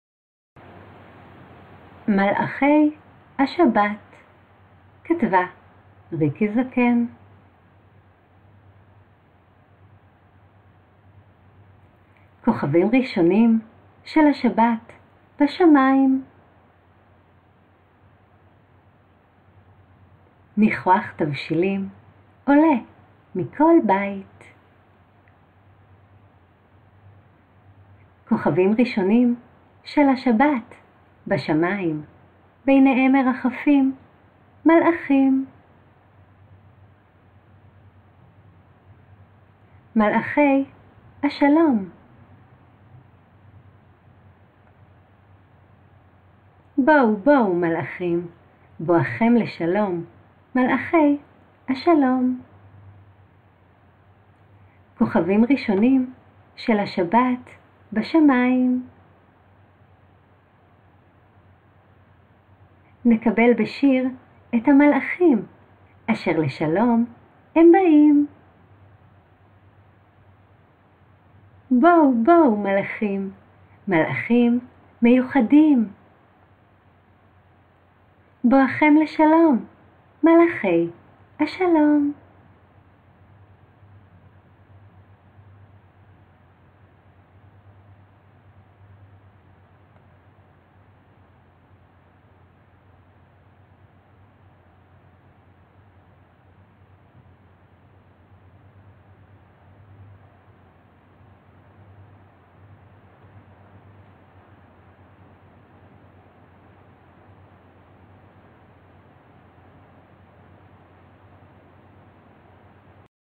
מלאכי השבת - סיפור לכבוד שבת - לילדים - כתבה: ריקי זקן
מלאכי השבת
סיפור ילדים לכבוד שבת